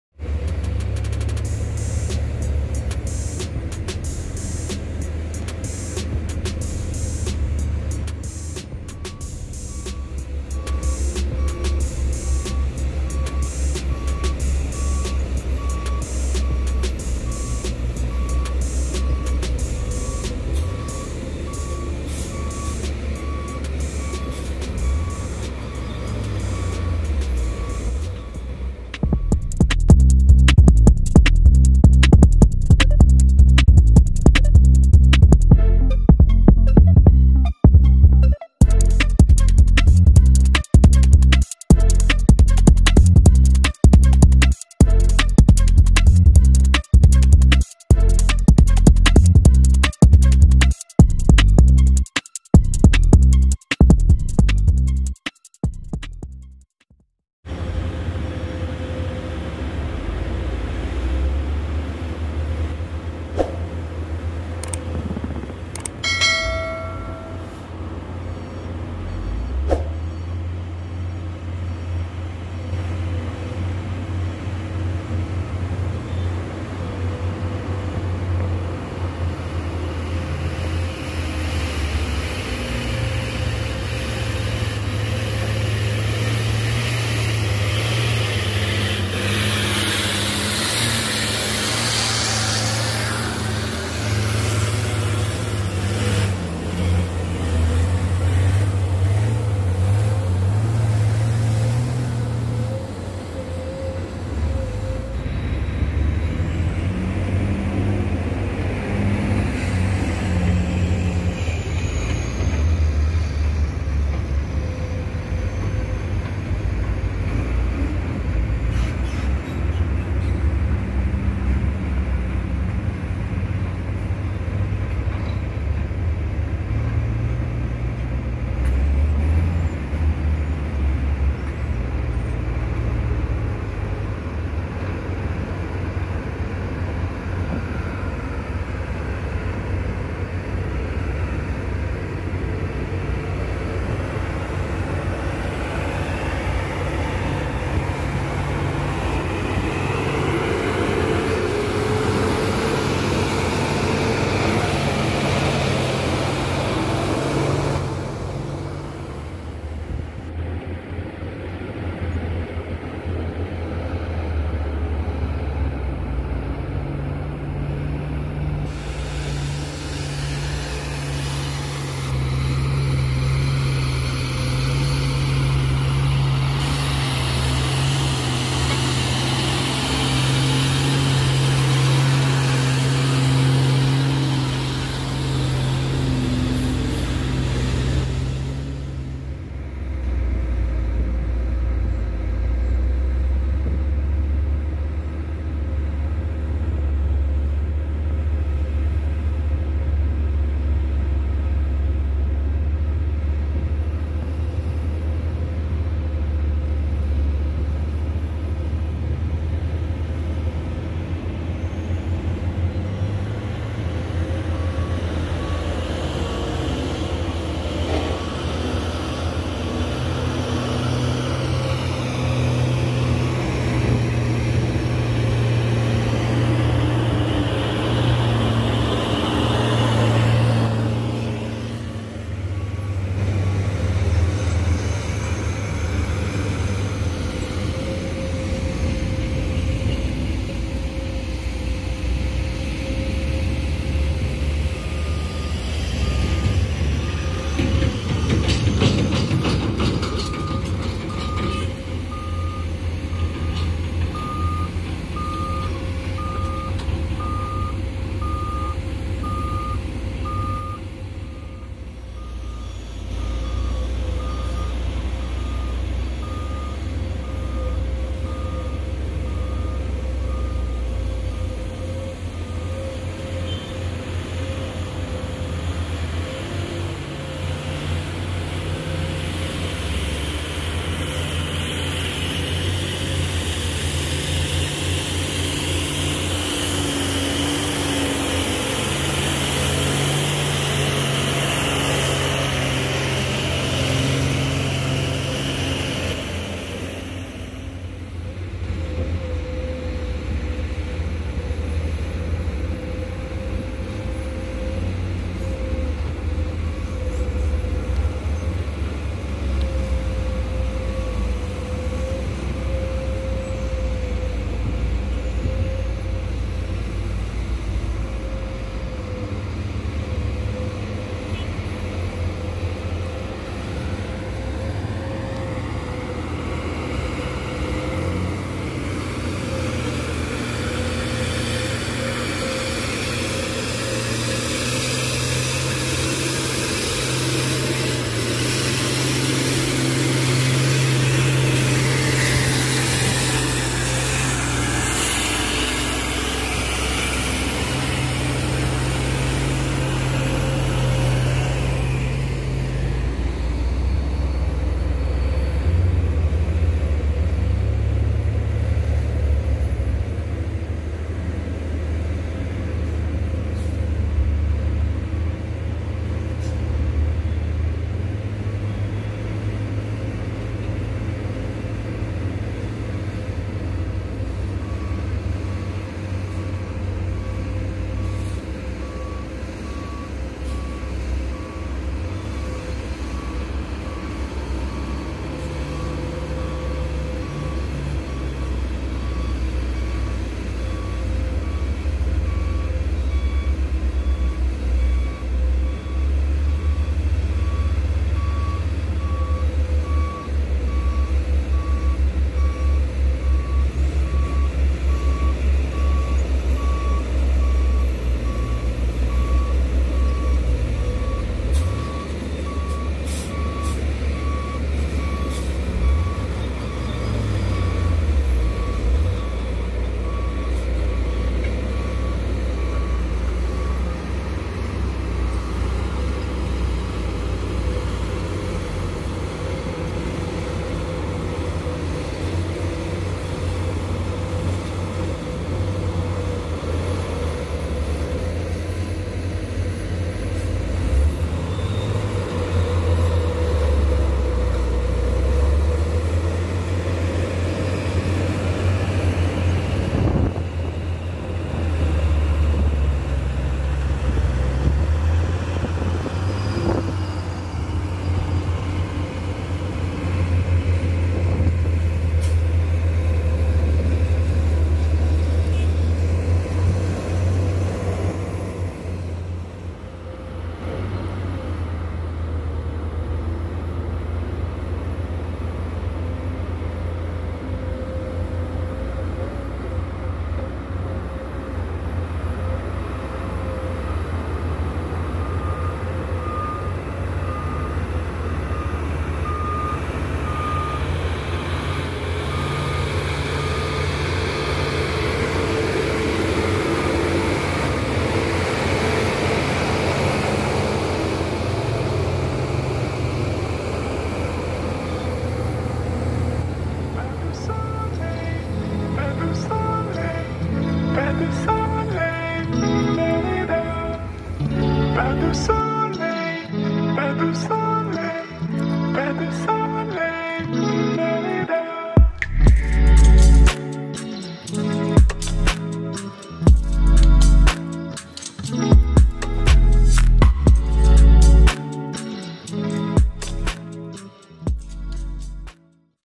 DUMP TRUCK HINO BERMUATAN TIDAK MAMPU MENANJAK
Assalamualaikum...
salam Otomotif
masih berada di lokasi tambang kali ini  kita akan menyaksikan unit dump truck hino pengangkut batu bara yang sedang menaiki tanjakan di jalan keluar pit dan salah satu di antada ada yang tidak mampu untuk menaikinya.
silakan di simak ya.